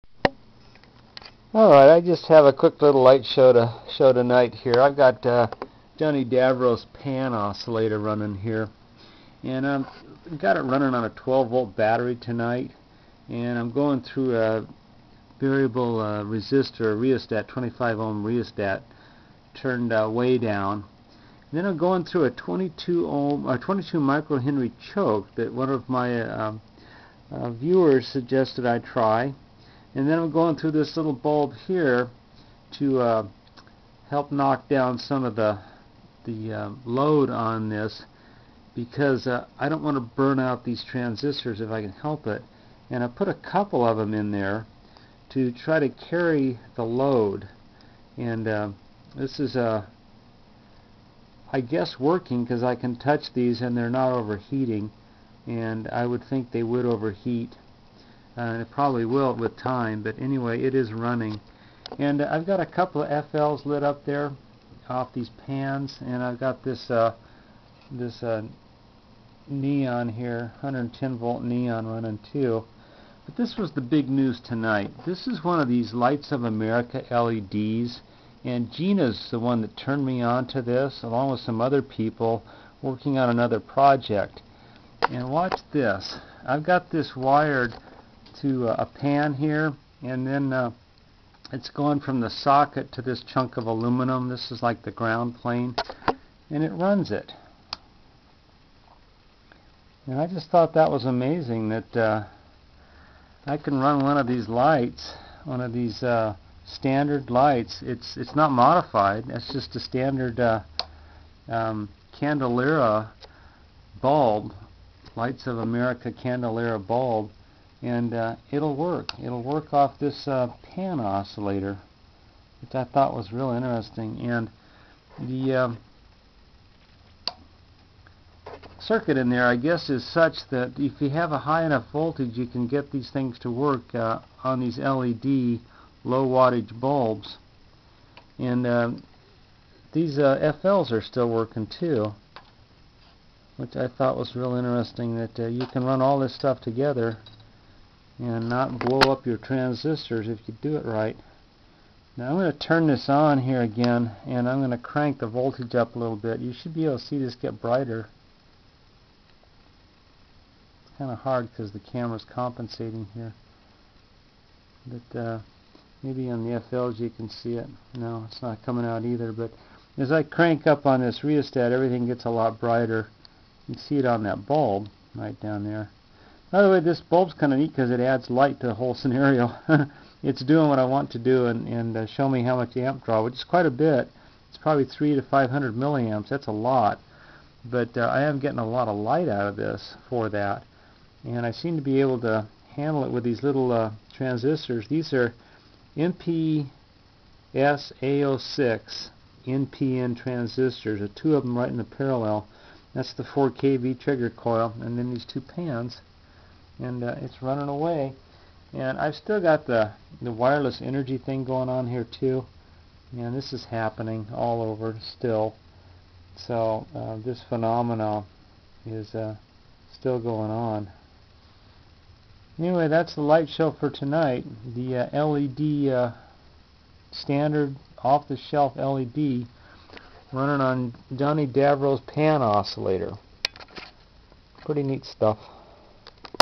Pan oscillator running 110 volt LED light
This is Jonnydavro's pan oscillator running a standard 110v /20 LED candelabra bulb.  In this video I am using a 12 volt battery with a rheostat to control the power.  Two small transistors are placed in parallel to help handle the load.  Two small fluorescent tubes and an a 110v neon are also being illuminated along with the LED bulb.  A small 3 volt filament bulb is in series with the battery to show the amp draw.